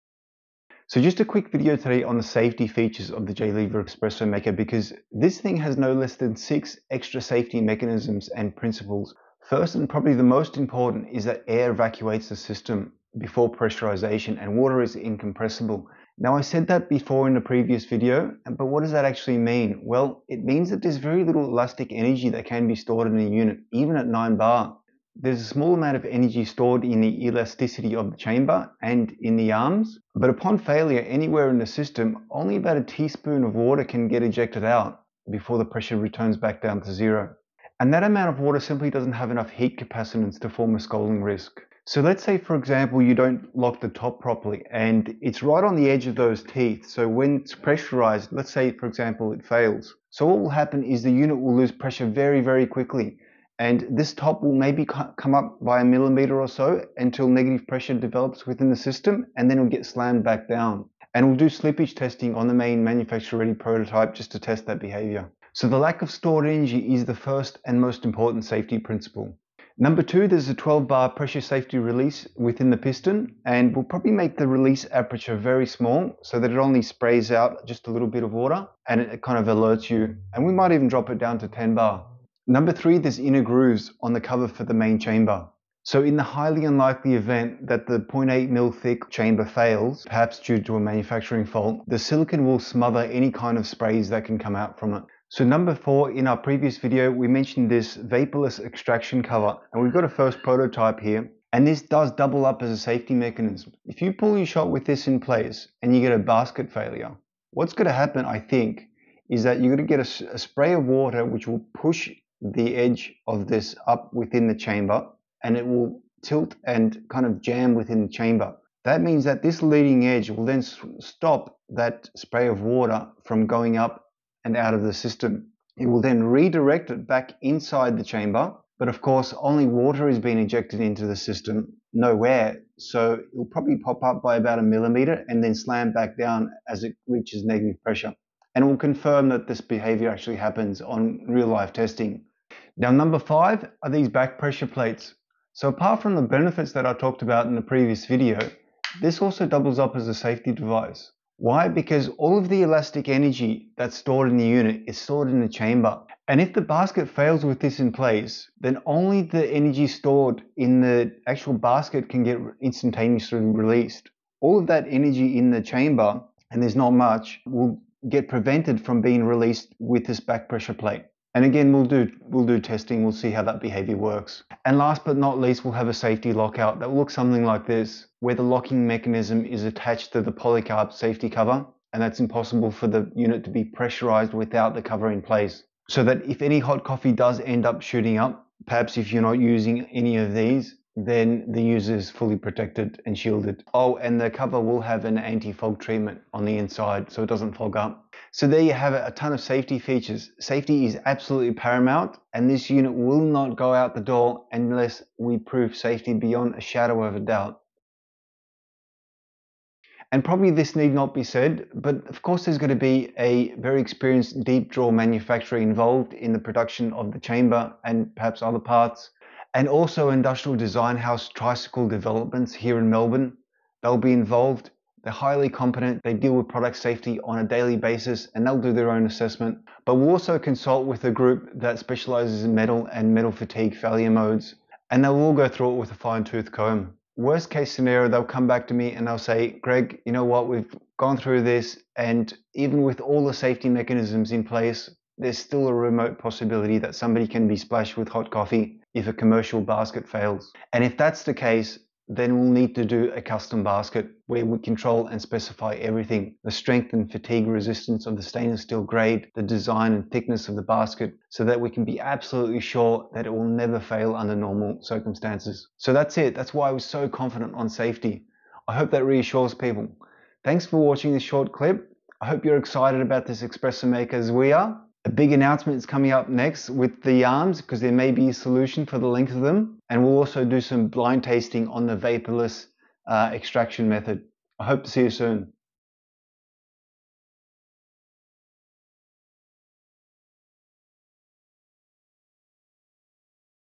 Quick Presentation on Safety Features of the Upcoming JLever58
Last of the technical videos, but we have to talk about safety, as I breezed through it in the intro video, and people wanted us to address it and the possibility of failed commercial baskets which are subject to fatigue after 100,000 shots.

Apologies for the poor audio, I had issues with the mic.

0:00 - Jlever58 safety features
3:31 - Independent testing
4:28 - Conclusion

If you have any questions/concerns, please let us know.